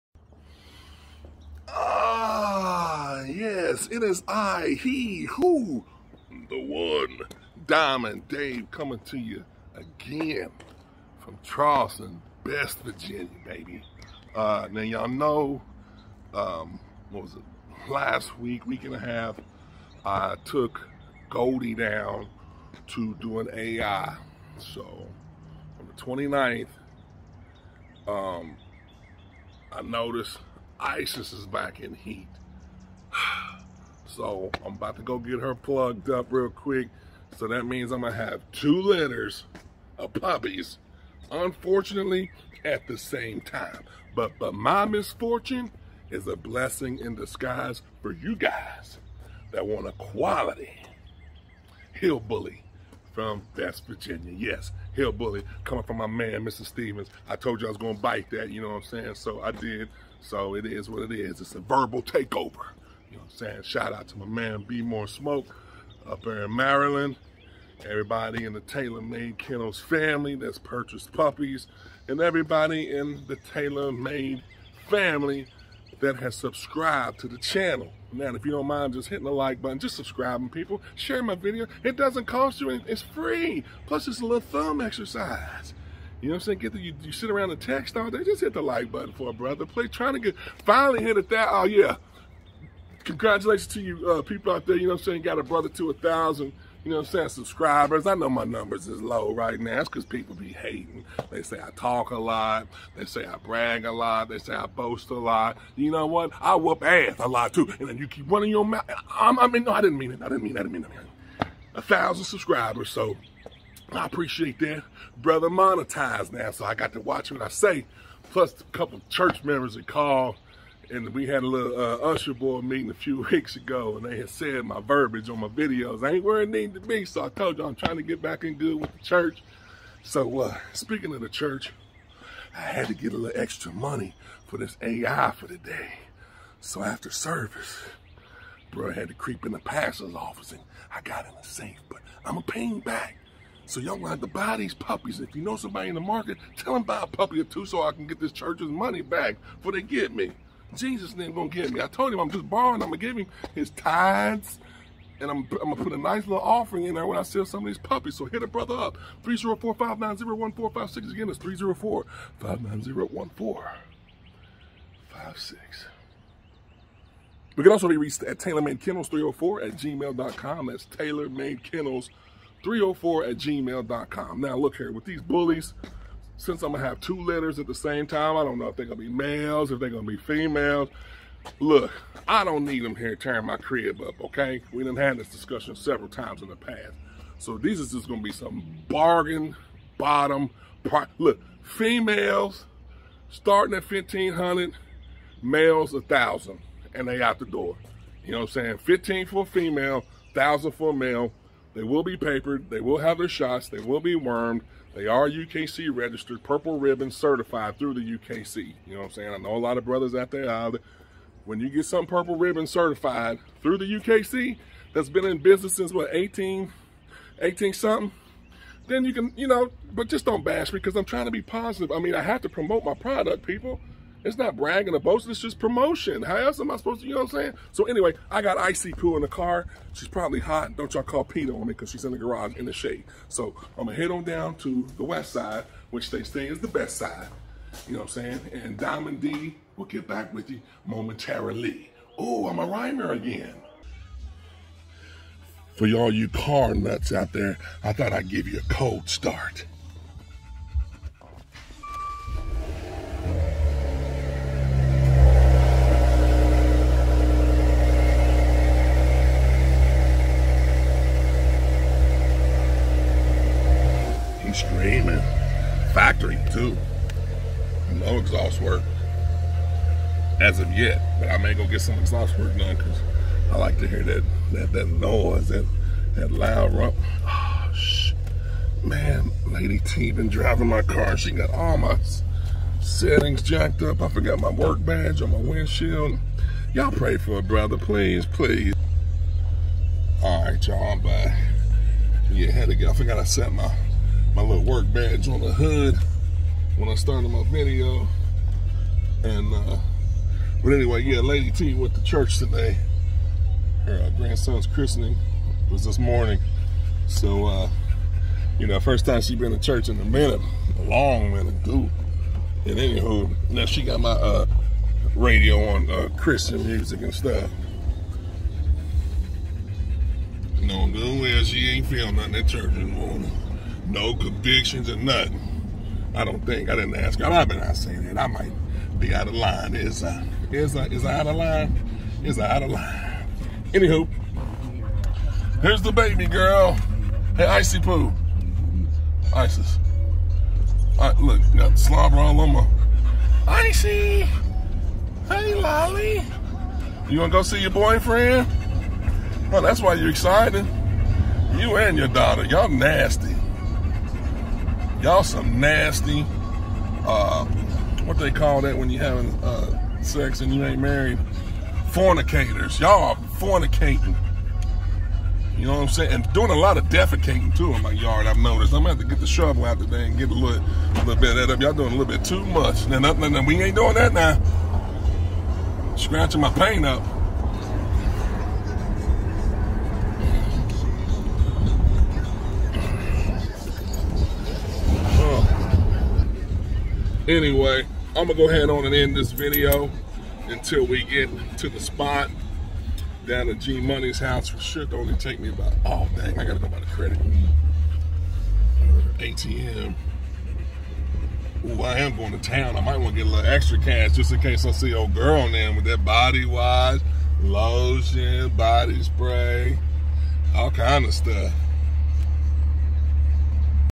American Hillbully breeding alert pt. 1
Isis is in heat again going to get another A.I. done. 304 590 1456 TaylorMade 💯